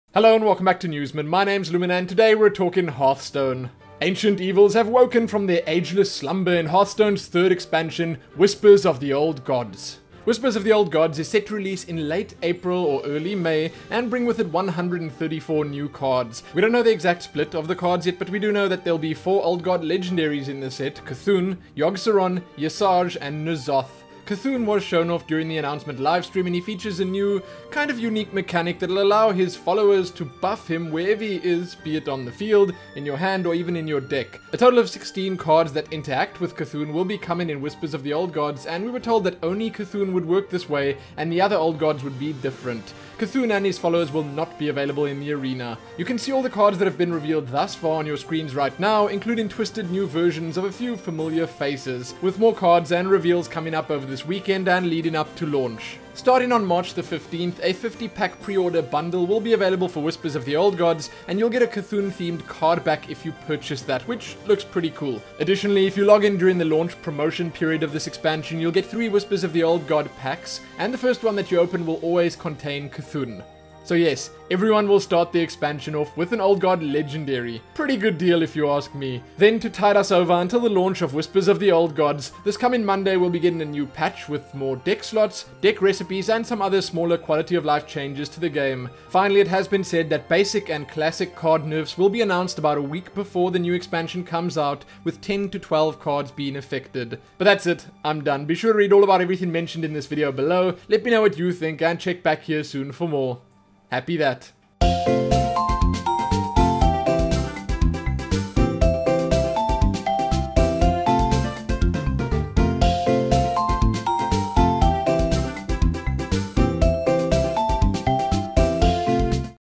Hearthstone: Whispers of the Old Gods Announced! (Newsmin 12/03/16)
In this episode of Newsmin we're looking at the announcement of Hearthstone's Whispers of the Old Gods expansion!

► Hearthstone

► Lumin Links ◄